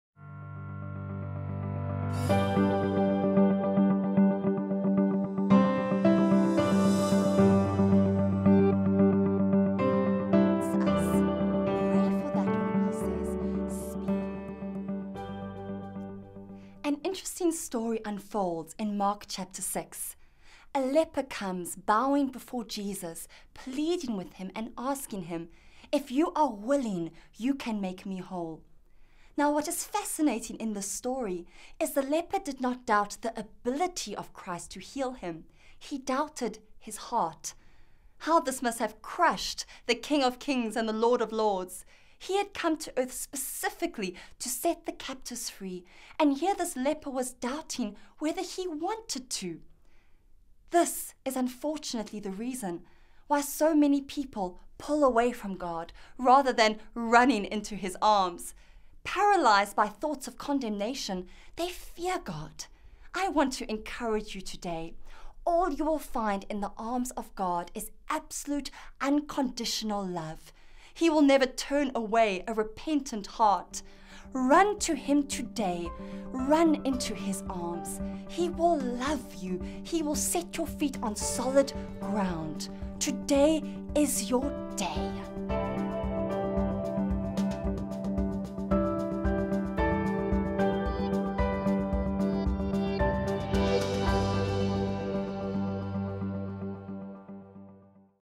10 Heart of God
Before any of His other roles or functions, God is Saviour! If you give Him an opportunity to rescue — by calling on the name of Jesus and asking Him to save you from your sins — He will do just that, no questions asked!